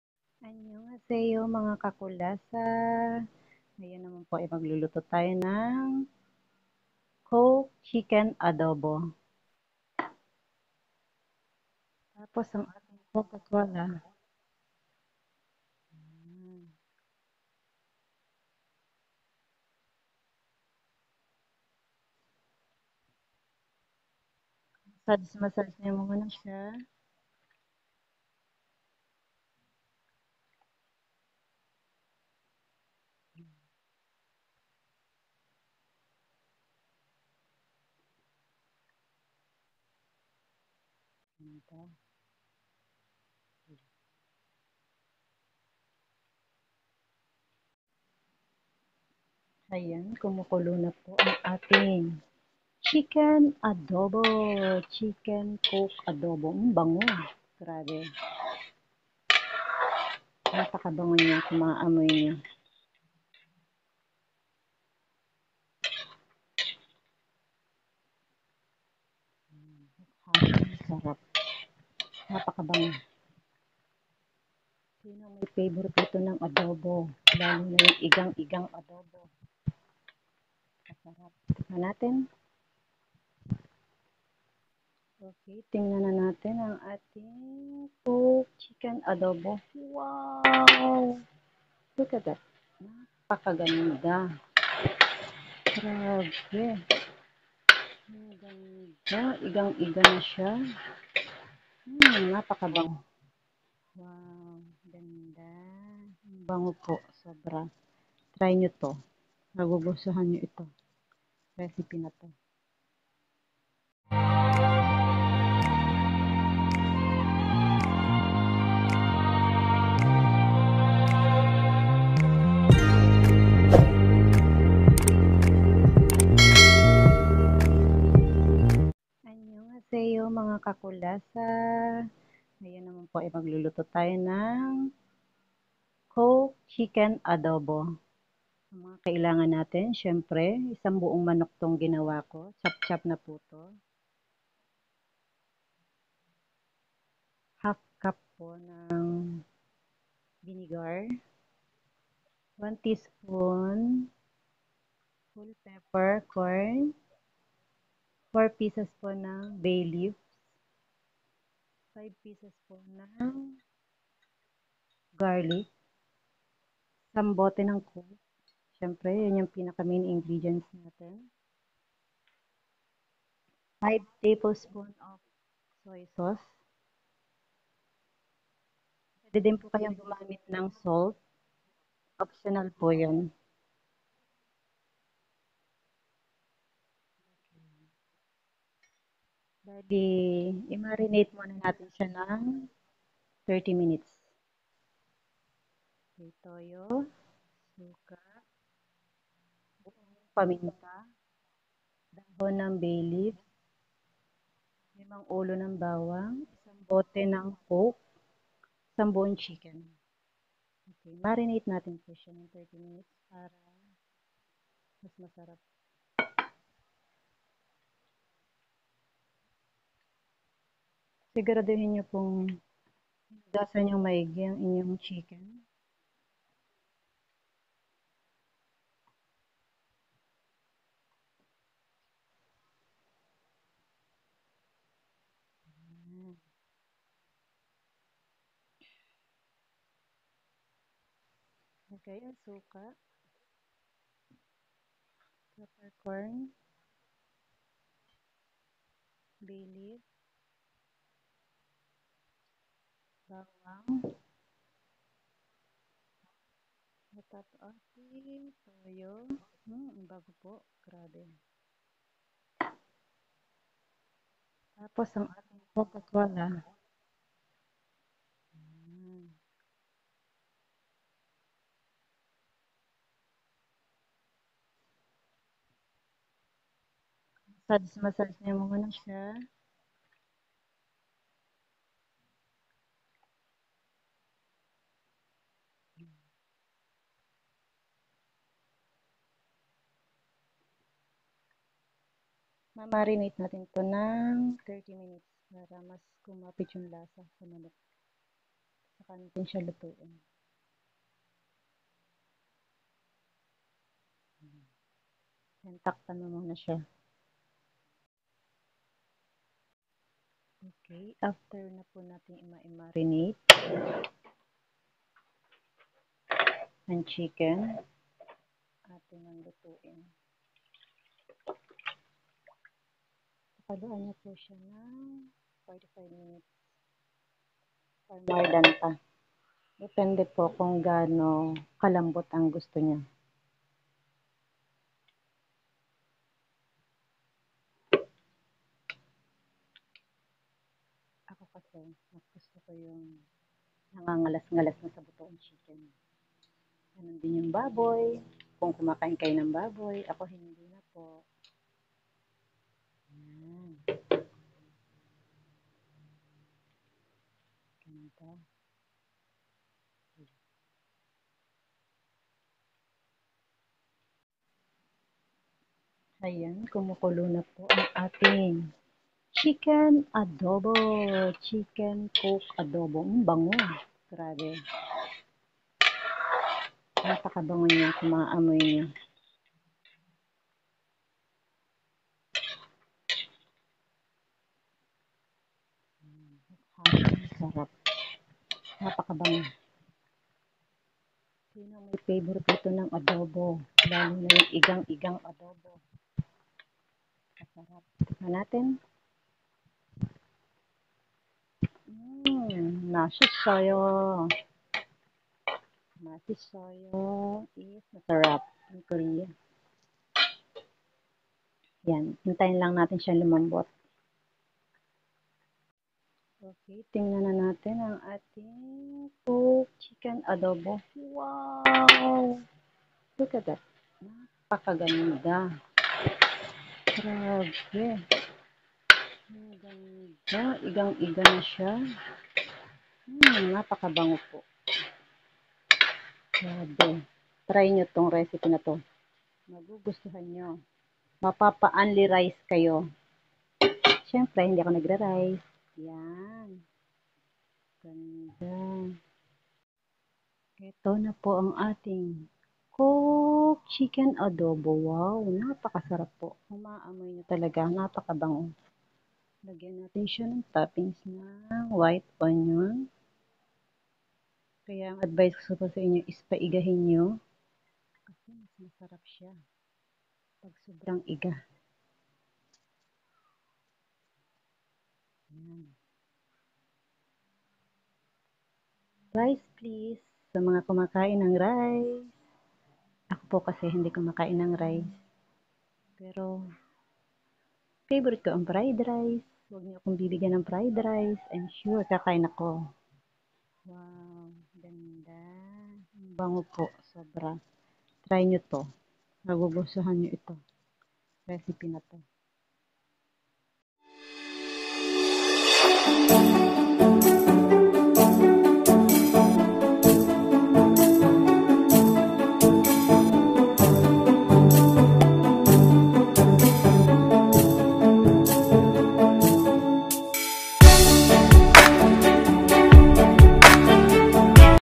BAKIT DIMO SUBUKAN ANG COKE CHICKEN ADOBO /// napaka simple lang gawin. try mo to magugustuhan mo
COKE CHICKEN ADOBO ingredients

1 WHOLE  CHOPPED
1 BOTTLE COKE
10 TBSP WHITE VINEGAR
5 TBSP SOY SAUCE
5 GLOVES GARLIC CRUSHED
2 TSP BLACK PEPPER
4 PCS. BAY LEAF
SALT( OPTIONAL)